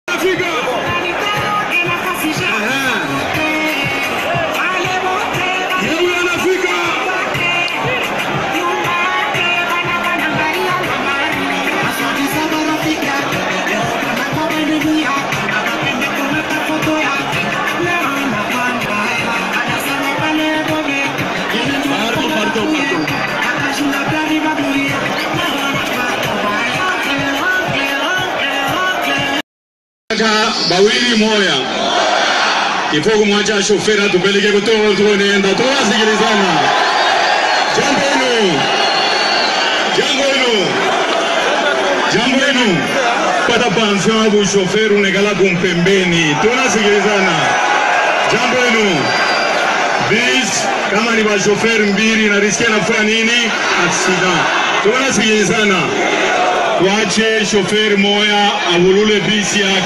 MOISE KATUMBI ALOBI NA FÉLIX TSHISEKEDI ATALA SIMA PE AZONGA MUTU
MOISE KATUMBI ALOBI NA FÉLIX TSHISEKEDI ATALA SIMA AZONGA MUTU